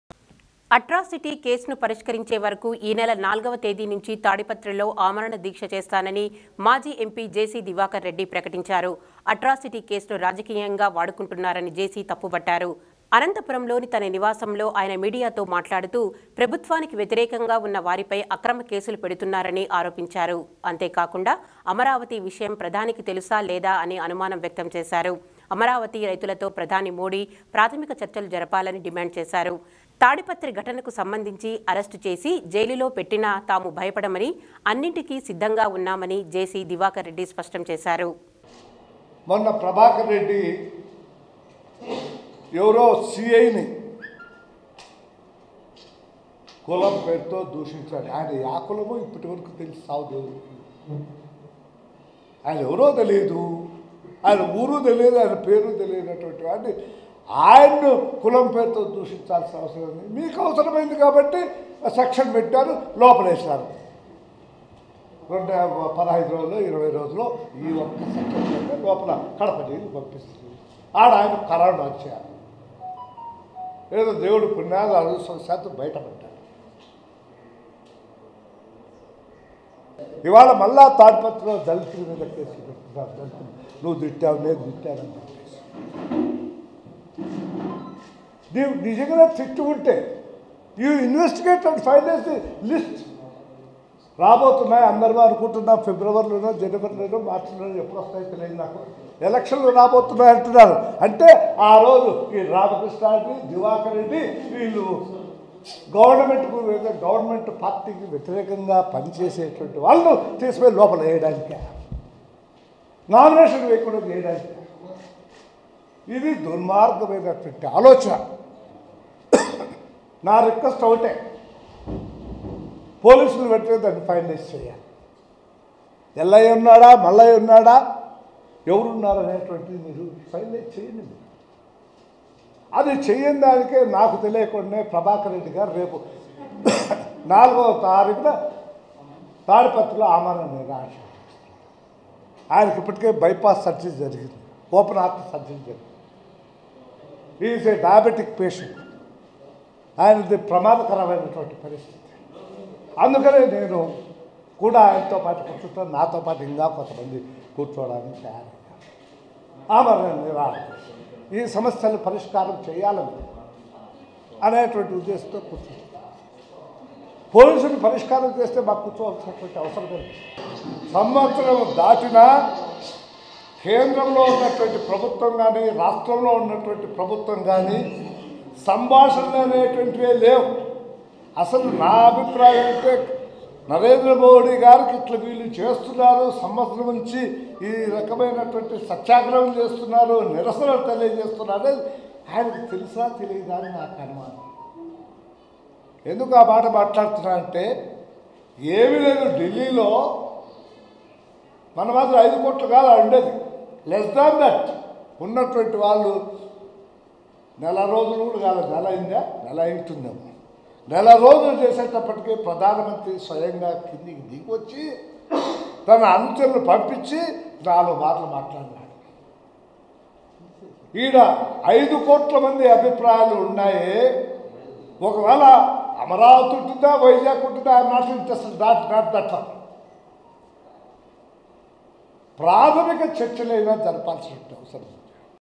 అట్రాసిటీ కేసును రాజకీయంగా వాడుకుంటున్నారన్న మాజీ ఎంపీ జేసీ దివాకర్‌ రెడ్డి
అట్రాసిటీ కేసును పరిష్కరించే వరకు... ఈ నెల 4 నుంచి తాడిపత్రిలో అమరణ దీక్ష చేస్తానని మాజీ ఎంపీ జేసీ దివాకర్‌ రెడ్డి ప్రకటించారు. అట్రాసిటీ కేసును రాజకీయంగా వాడుకుంటున్నారని జేసీ తప్పుబట్టారు.